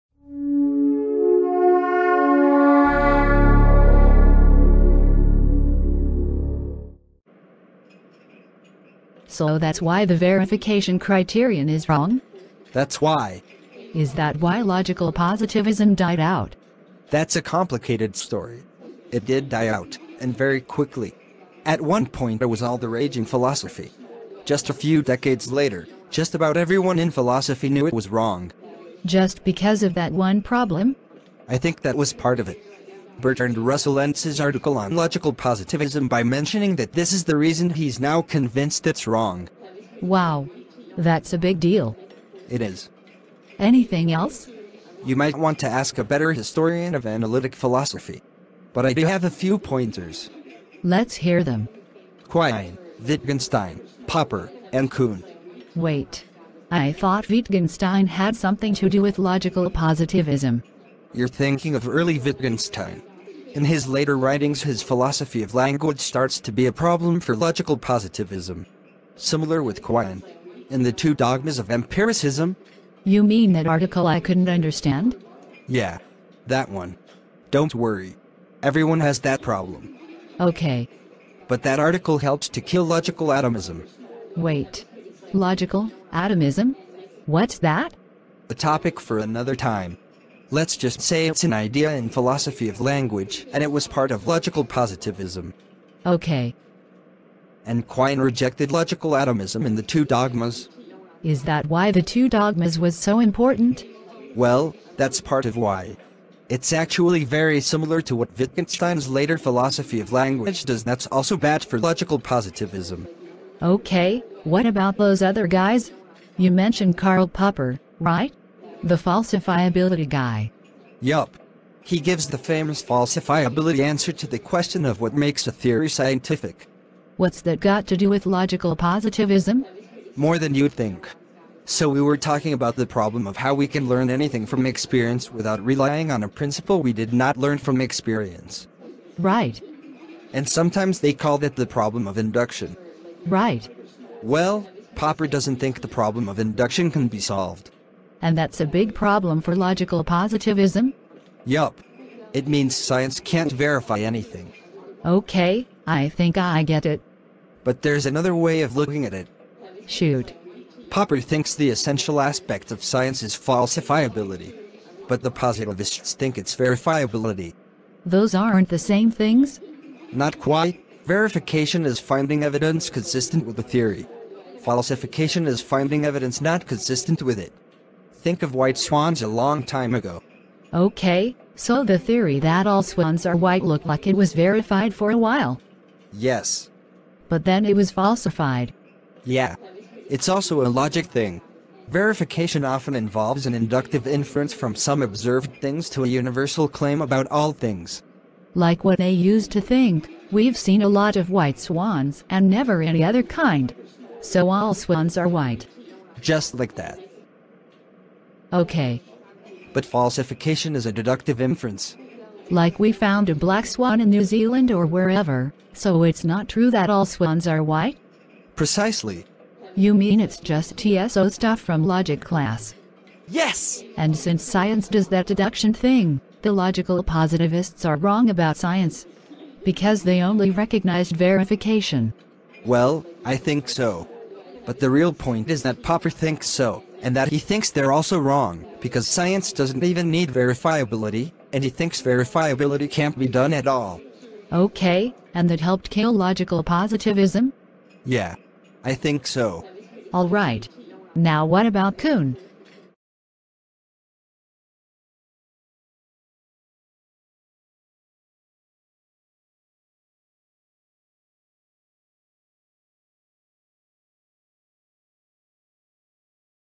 Part 4: What Killed Logical Positivism?
Why did Logical Positivism die? Also Quine, Wittgenstein, and Popper!